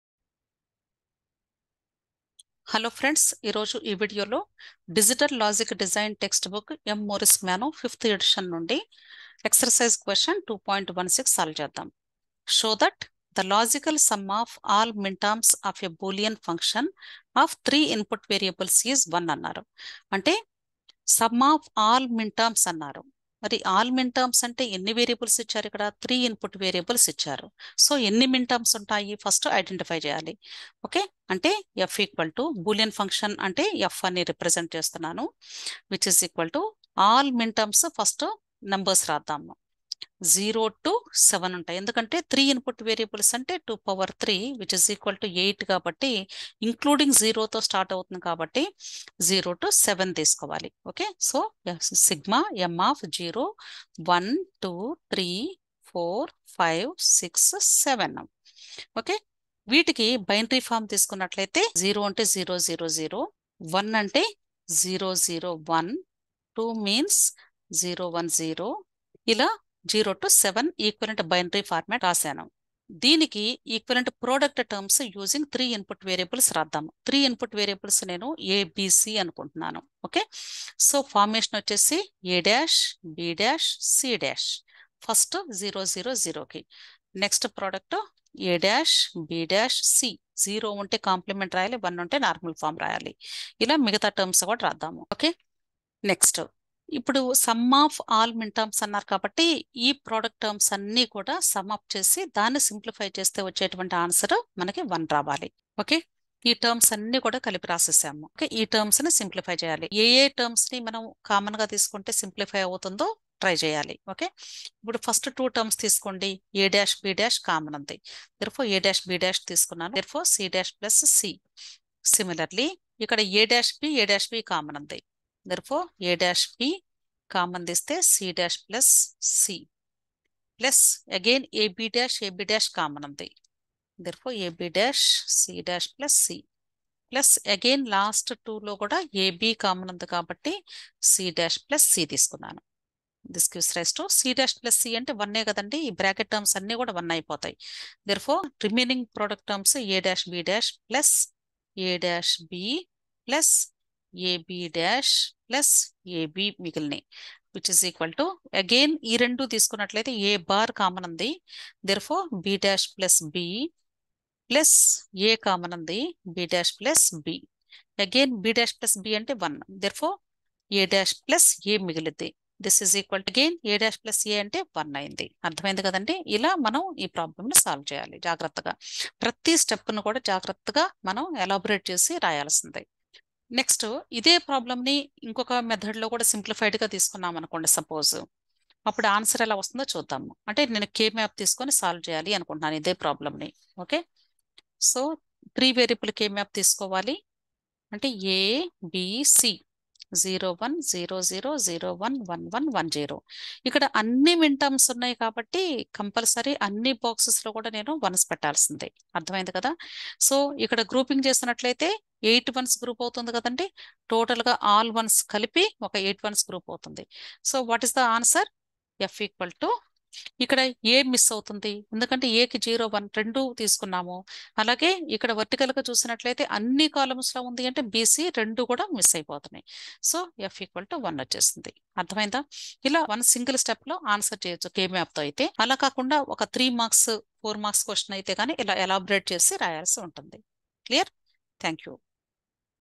2.16 The logical sum of all minterms of a Boolean function of n variables is 1. (a) Prove the ...
✅This video provides the solution to the problem:
2.16 The logical sum of all minterms of a Boolean function of n variables is 1.
 (a) Prove the previous statement for n = 3.
 (b) Suggest a procedure for a general proof.